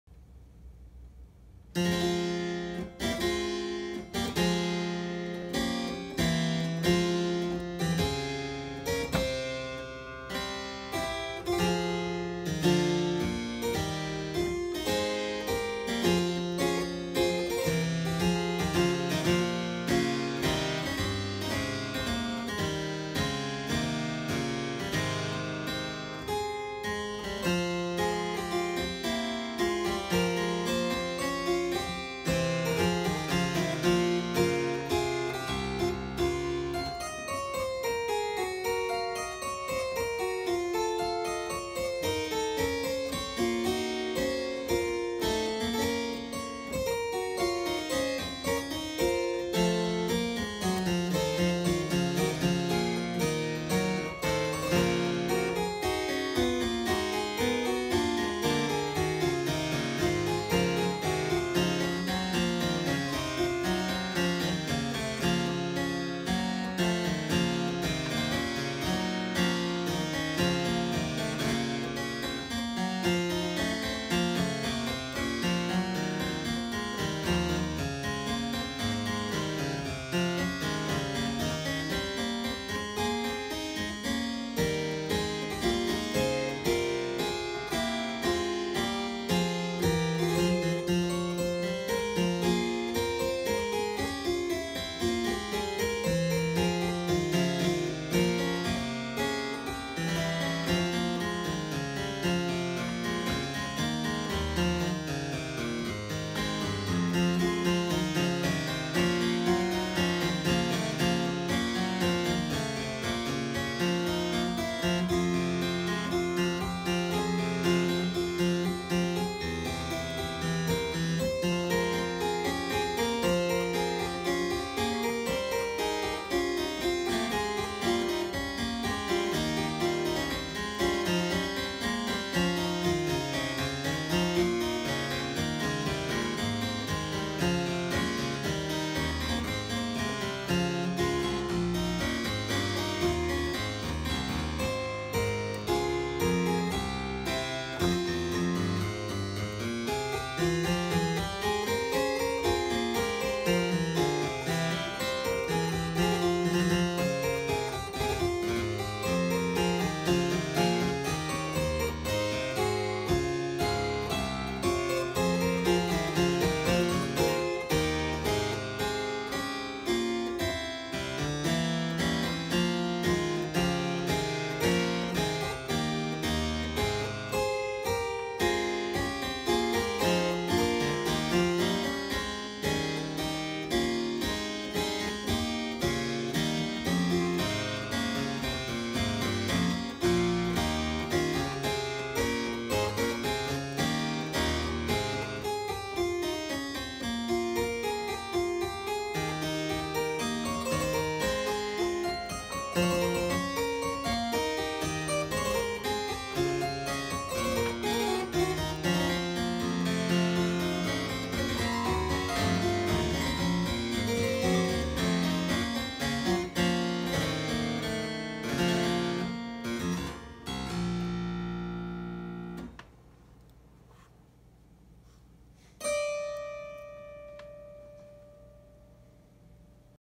Interrupted by the cat… AGAIN!
Improving in some minor keys, only for the cat (Tiny) to put his own personal touch at the end. Ahhhh, the life of a cat dad 🐱